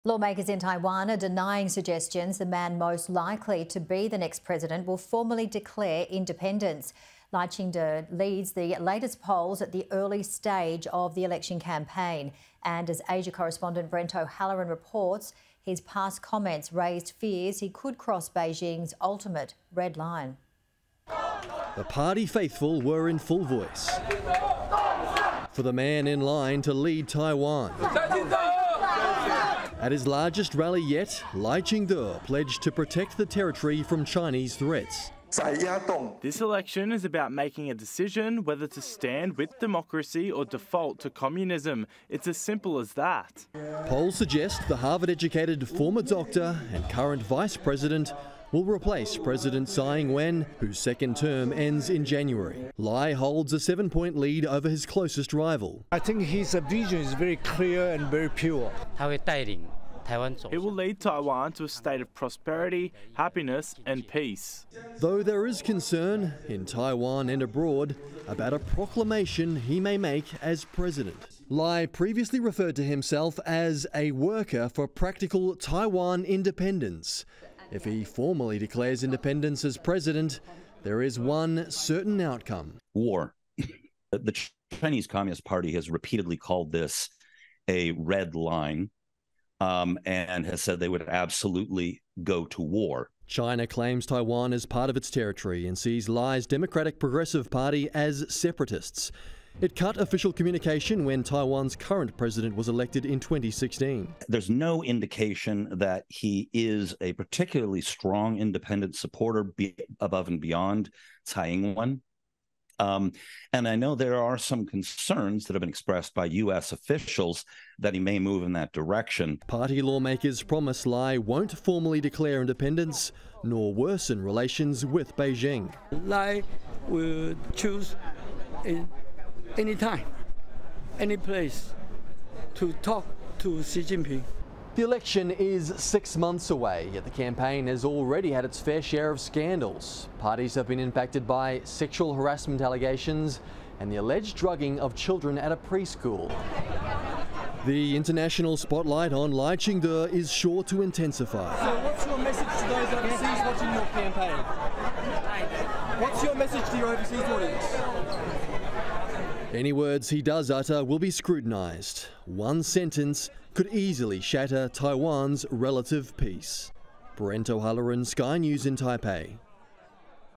Lawmakers in Taiwan denying suggestions next President will declare dependence
Lawmakers in Taiwan are denying suggestions the man most likely to be the next President will formally declare dependence.